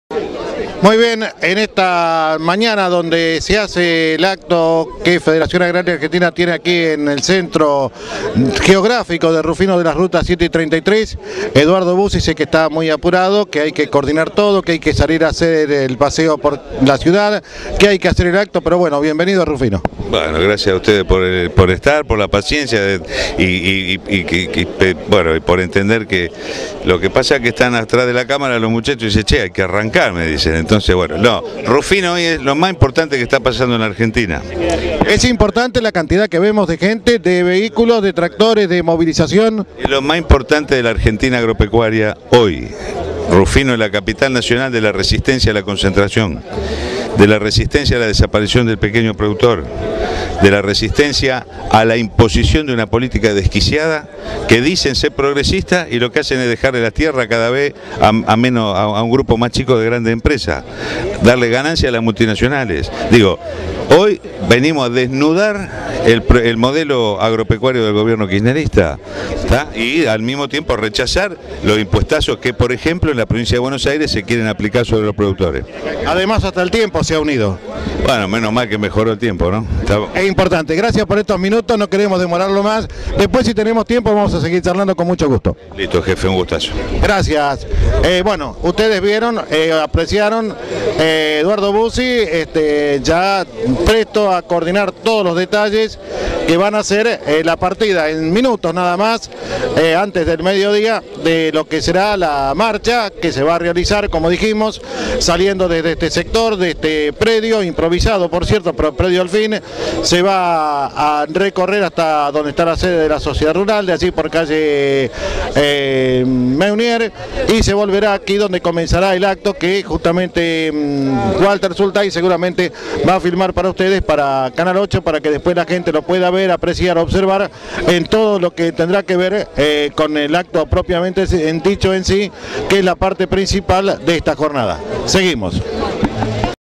Eduardo Buzzi en Rufino
Eduardo Buzzi presidente de Federación Agraria Argentina, en diálogo con Néstor Concaro, el miércoles 23 de mayo, en rotonda de los cruces de rutas 7 y 33, con motivo del Tractorazo, acto y movilización.

Canal 8 Rufino